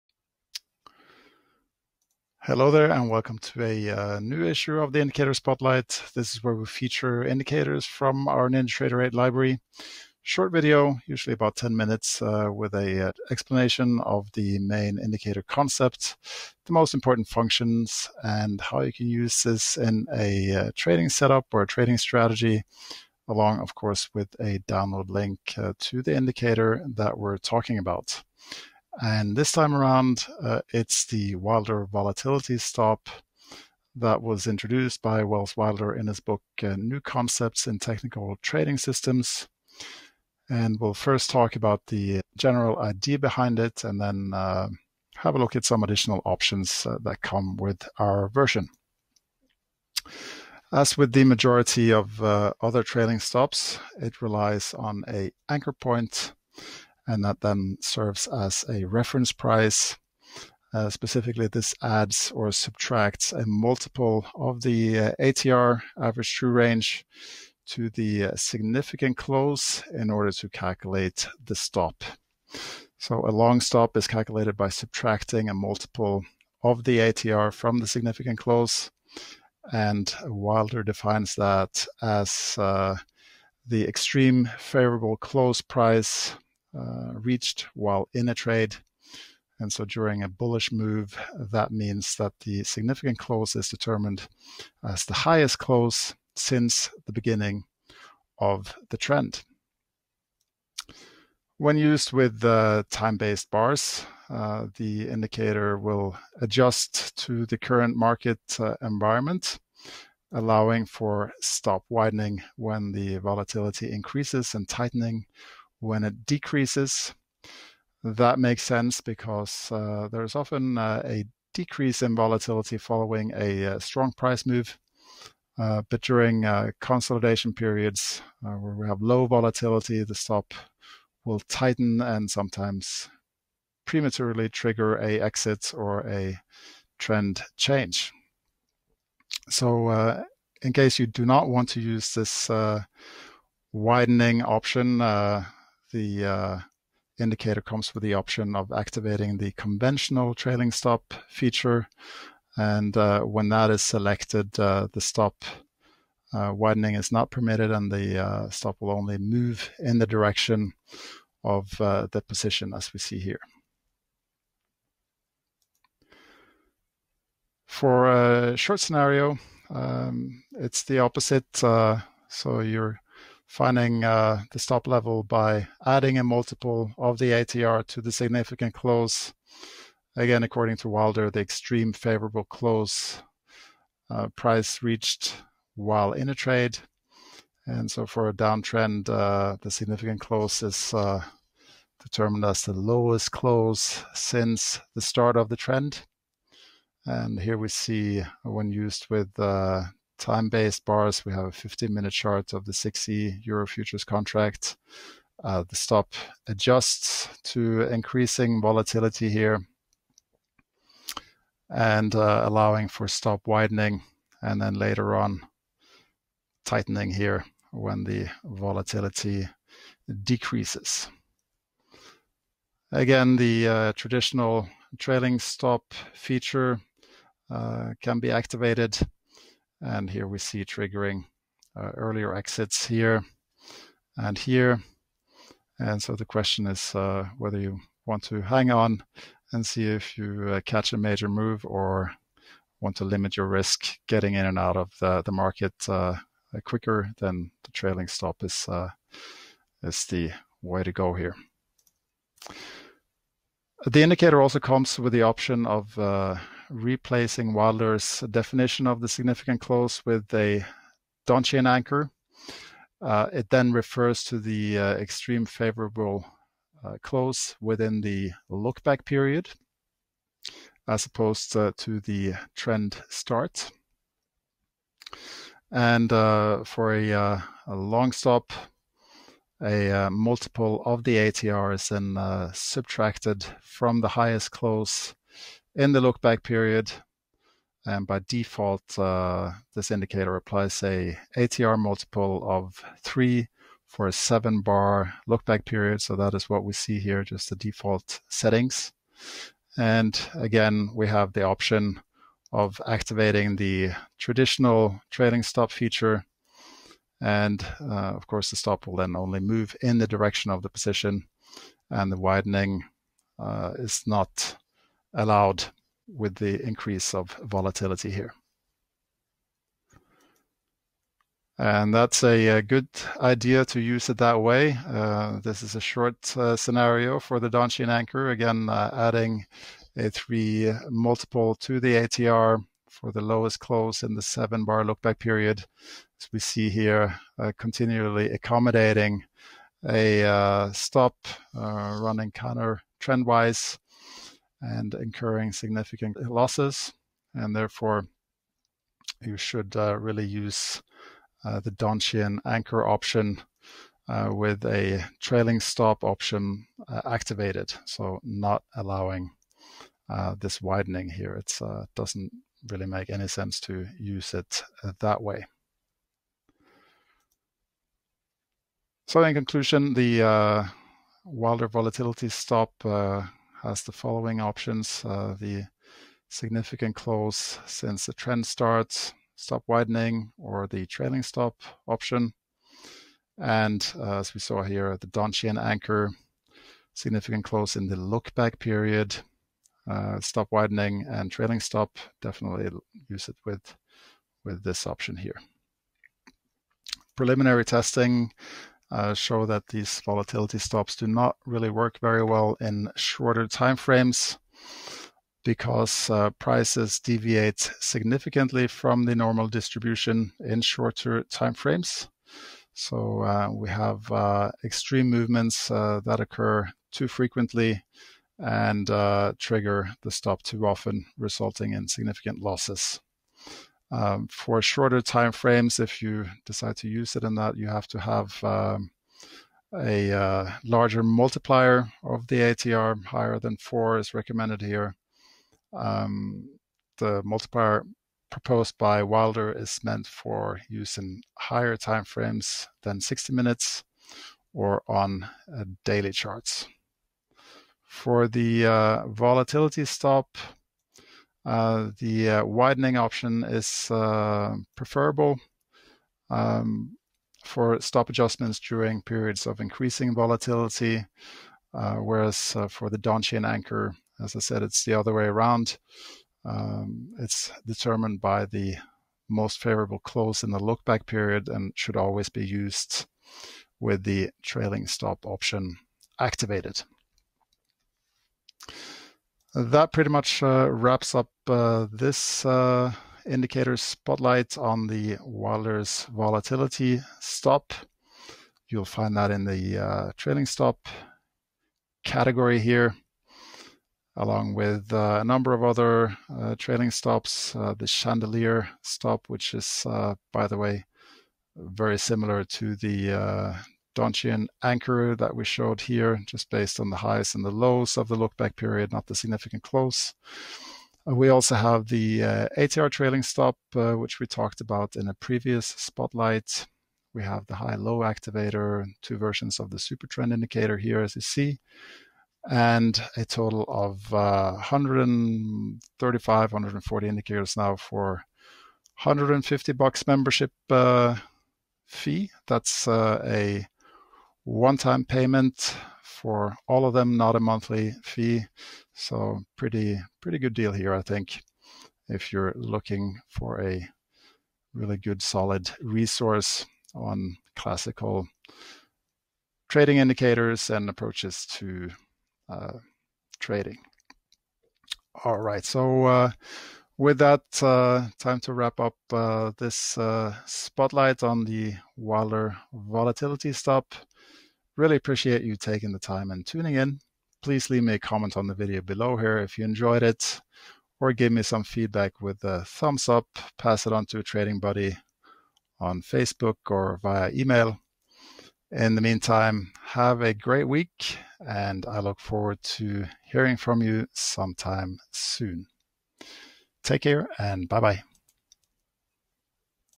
Volatility Stop Indicator Spotlight
00:00 Spotlight Intro
00:32 Wilder Volatility Stop indicator concept
01:20  A Significant Close
01:45 Adjusting to increasing / decreasing volatility
03:20 Stop widening vs. stop tightening
06:37 Preliminary test results
08:08 Other Library Stop Loss indicators

This volatility stop indicator was introduced by Welles Wilder in “New Concepts in Technical Trading Systems”. As with the majority of other trailing stops, it relies on an anchor point which serves as a reference price. Specifically, it adds or subtracts a multiple of the Average True Range (ATR) to the “significant close” in order to calculate the stop. When used with time based bars, the Volatility Stop indicator adjusts to the current market environment. Specifically, it allows for stop widening when volatility increases and tightening when it decreases.
A blogpost discussing the the Wilder Volatility Stop can be found